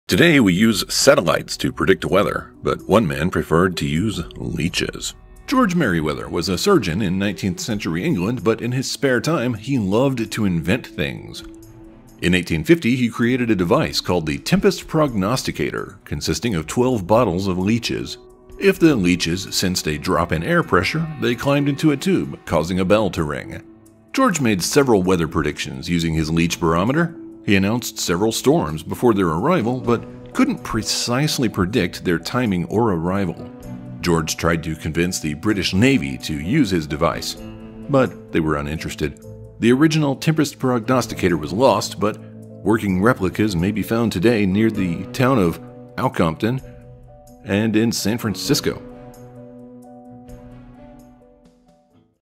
The Leech Barometer
In 1850 an inventor named George Merryweather created a strange device.  It was called the Tempest Prognosticator and used leeches to detect a change in air pressure.   When the leeches climbed, it would cause a bell to ring.    George predicted several storms but he couldn't tell when they would arrive or where.   He tried to convinced the British Royal Navy to use his creation but they were uninterested.